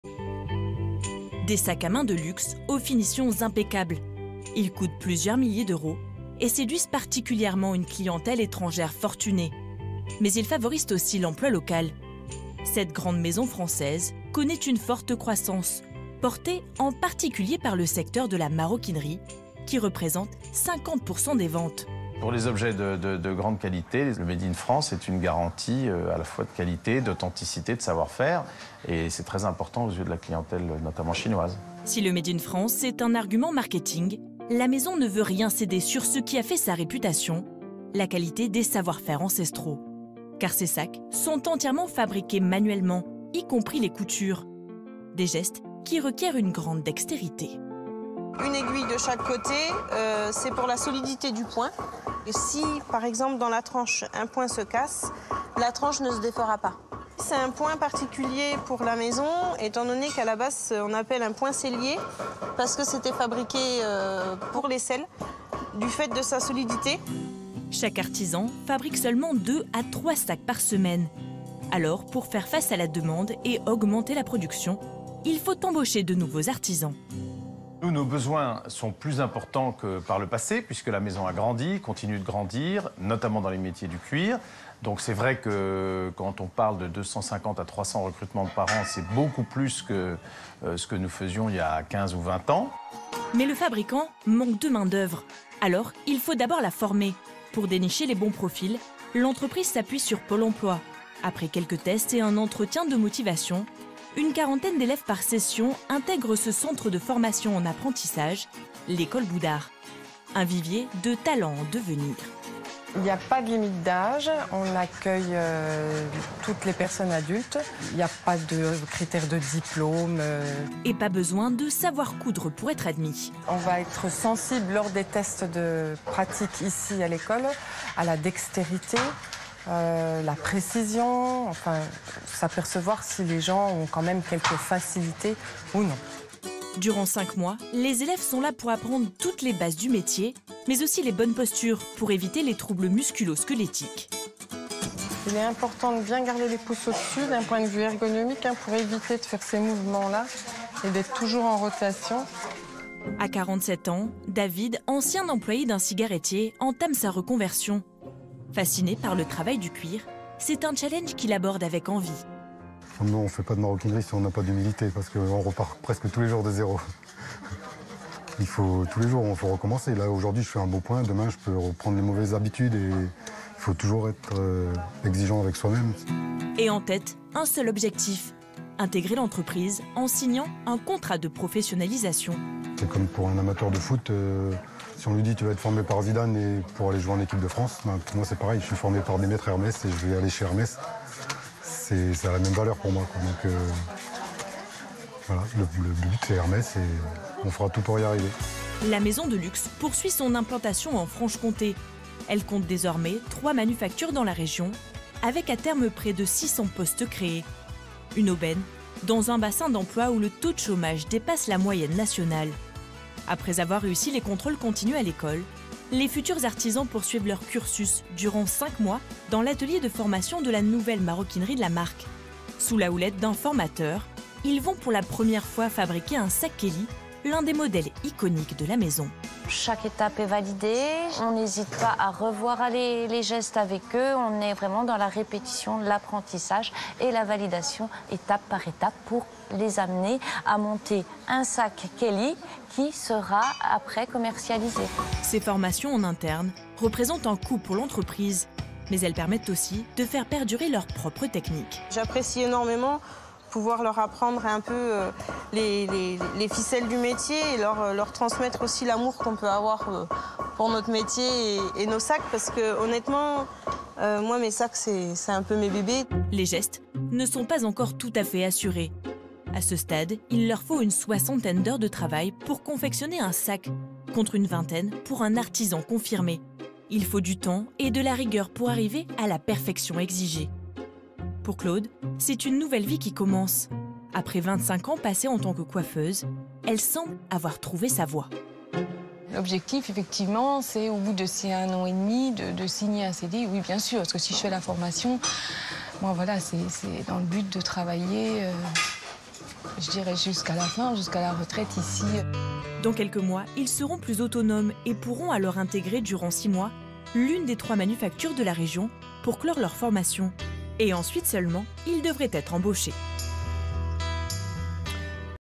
Dans les coulisses d'une maison de luxe qui forme ses artisans
La maroquinerie de luxe est un secteur qui ne connaît pas la crise. A tel point que certaines grandes maisons continuent d’ouvrir de nouvelles manufactures. Près de 3000 artisans français perpétuent déjà des savoir-faire ancestraux mais il reste encore des centaines d’emplois à pourvoir. Alors quels sont les critères de sélection ? Comment ces orfèvres du cuir sont-ils recrutés et formés ?